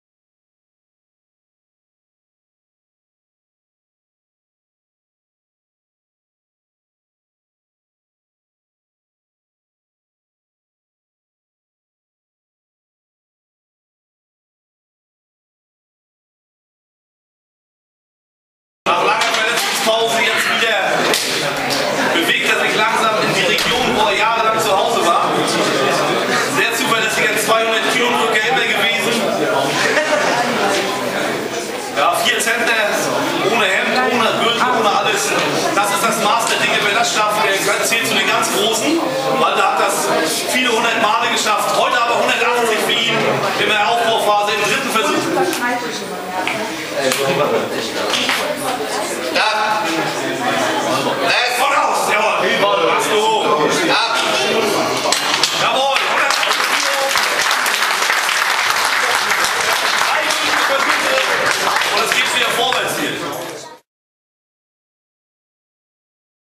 benchpress 180 kg - in a hopeful way on the road to 200 kg again - Walter Kurda
G-Raw-A benchpress-meeting, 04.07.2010 city of Leverkusen, GER